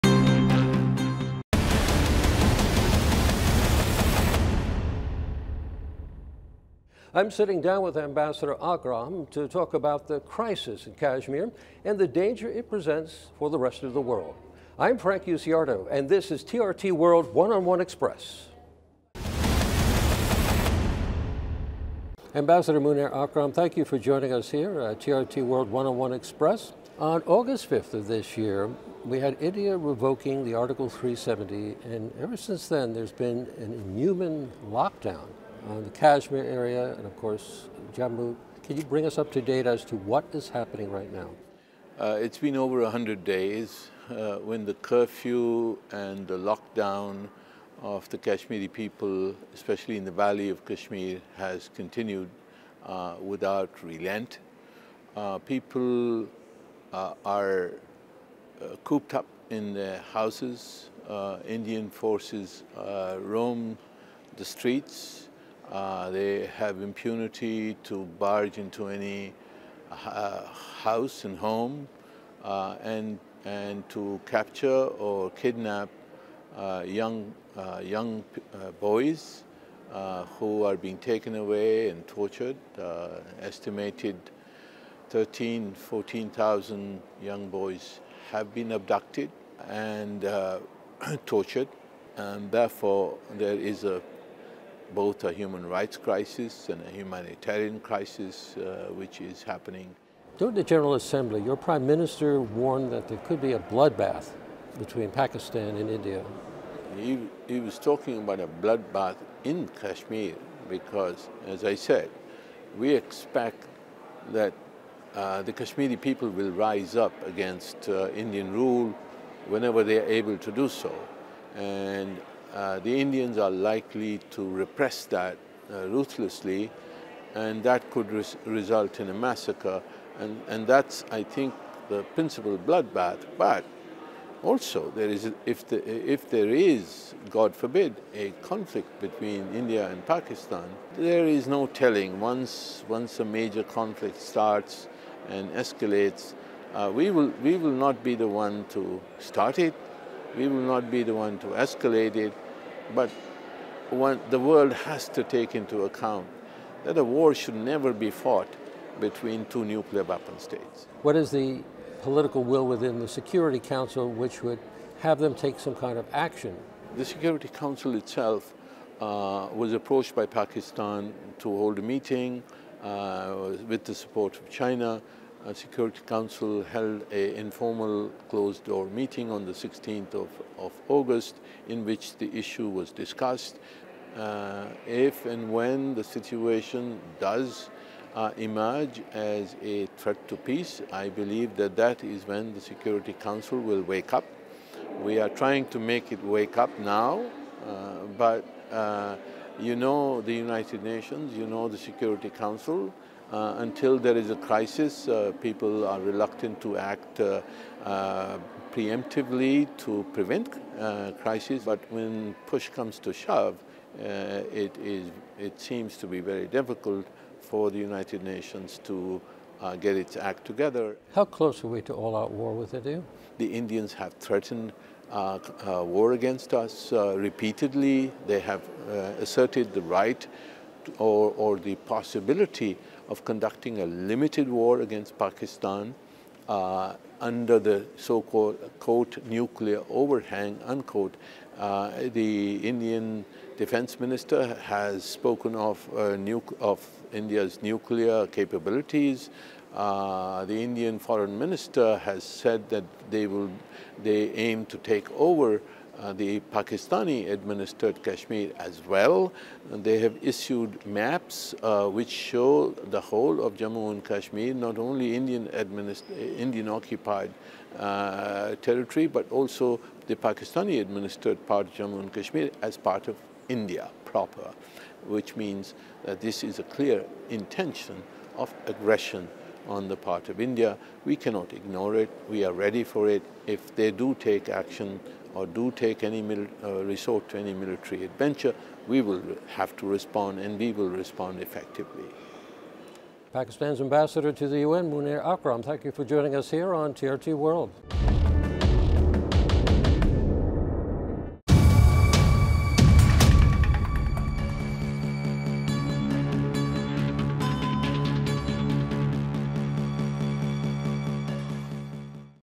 One on One: Exclusive interview with Munir Akram, Permanent Representative of Pakistan to the UN
We made an interview with Munir Akram, Permanent Representative of Pakistan to the UN on the Kashmir tension.